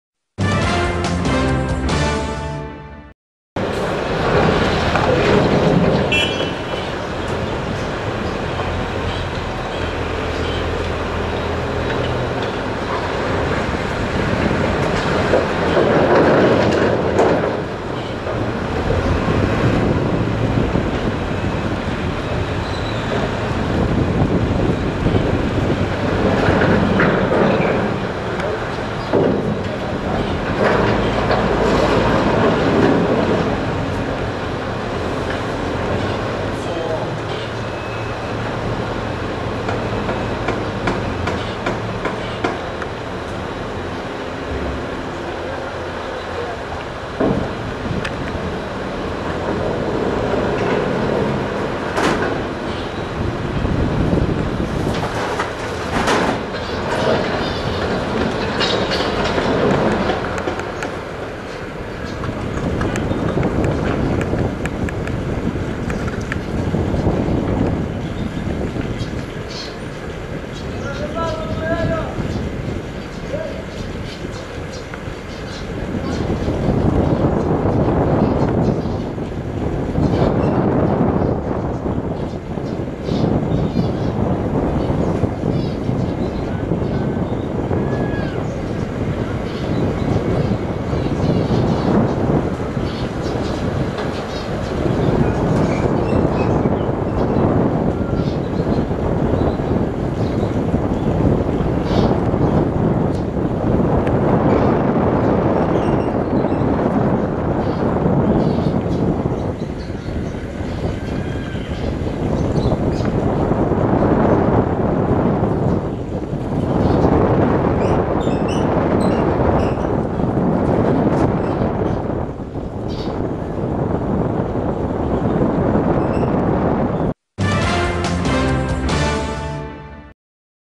قوات النظام برفقة جرافات يزيلون الحواجز التي يحتمي بها الثوار بسترة مهزة 16/12/2013 Bahrain
قوات النظام برفقة جرافات يزيلون الحواجز التي يحتمي بها الثوار بمنطقة منطقة مهزة بجزيرة سترة جنوب العاصمة المنامة صباح اليوم الأثنين (16 ديسمبر/كانون الأول 2013) بالتزامن مع إنطلاق الجولة الأولى تحت شعار " يسقط حمد " تأهباً لتظاهرة الموحدة وسط العاصمة المنامة يوم الغذ الثلاثاء